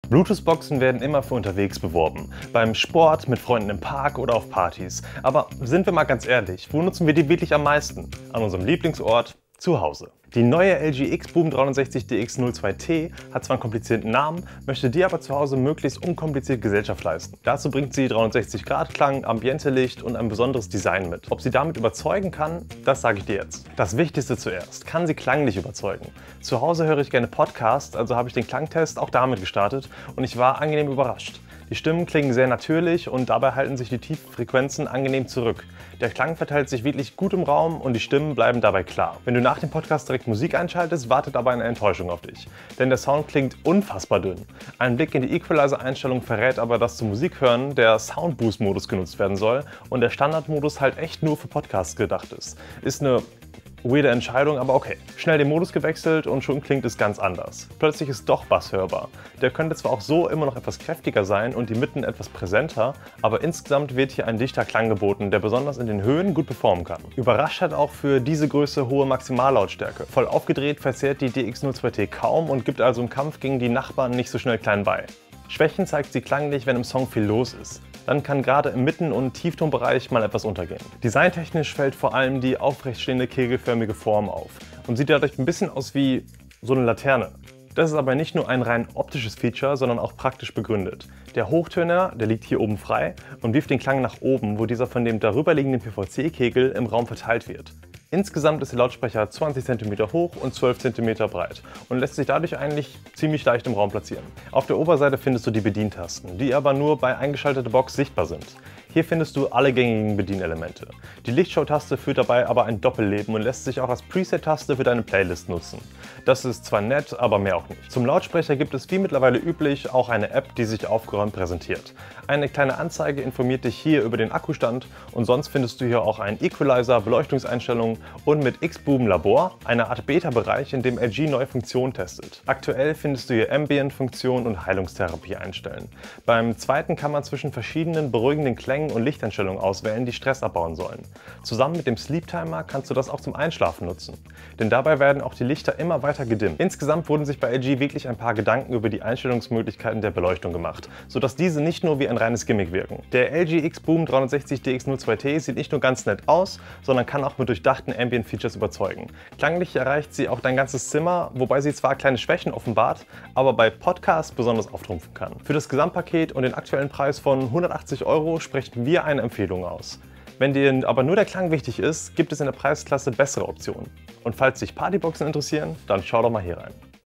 Sounds zum Entspannen! LG XBOOM 360 DX02T
0:00 | Intro
0:24 | Klang
1:21 | Design
1:45 | Features
2:34 | Fazit

Weitere Informationen zum Thema Soundbars, Heimkino-Anlagen, Surround Sound und vielen weiteren Themen rund um den Bereich Unterhaltungselektronik findest du